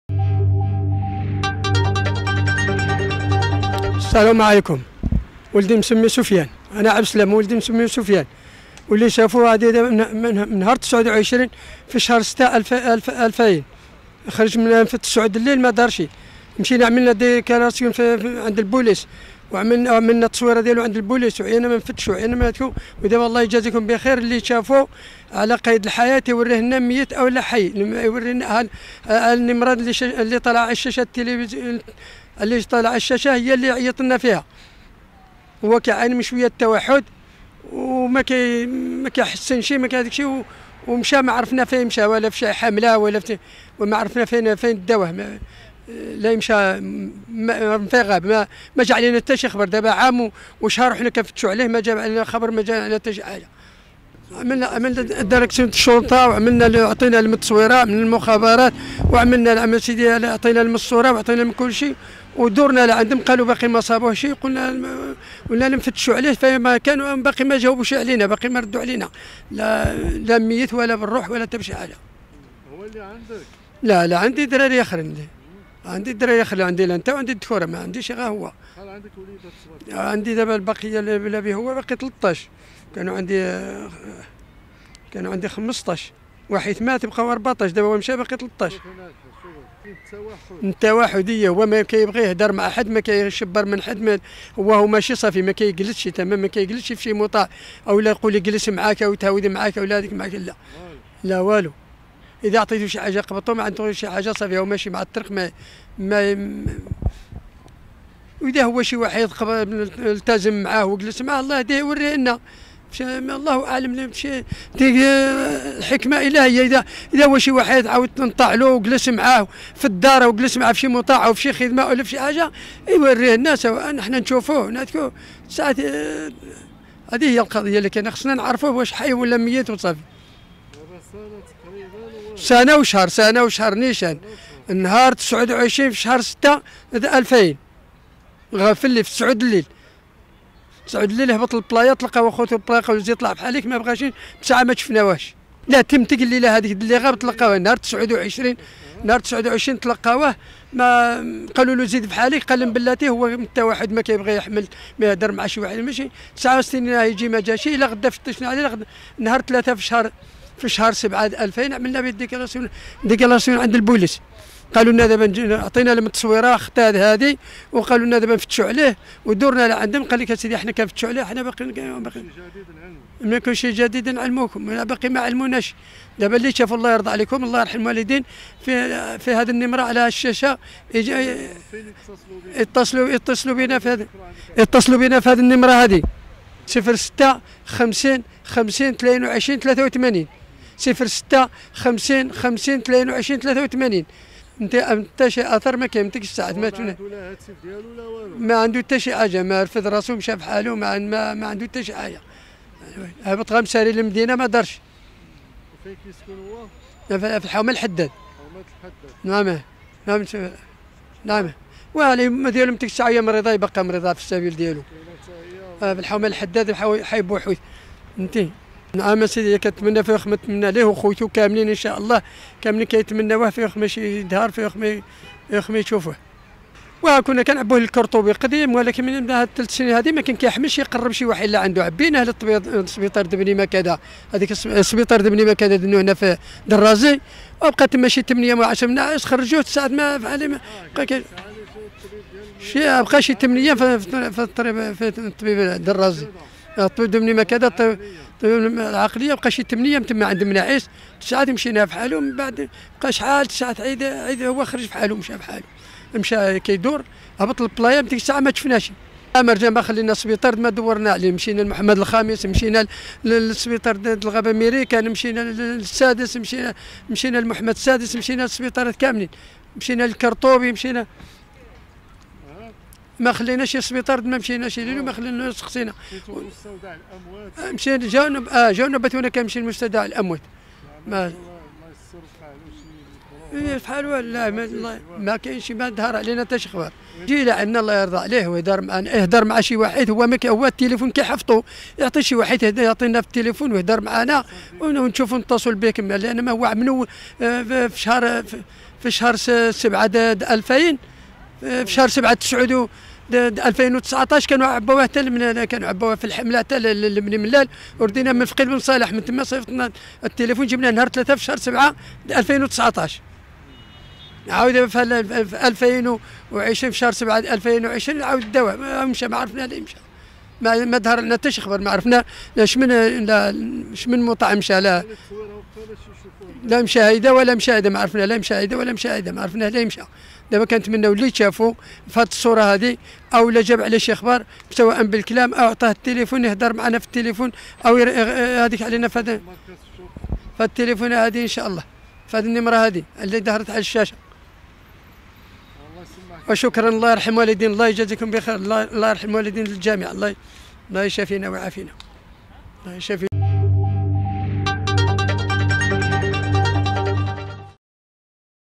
اختفاء شاب مصاب بالتوحد بطنجة والأب المنهار في صرخة قوية:"عاونوني نلقا ولدي"
كشف أب عن تفاصيل خاصة باختفاء ابنه المصاب بالتوحد، داعيا إلى مساعدته في البحث.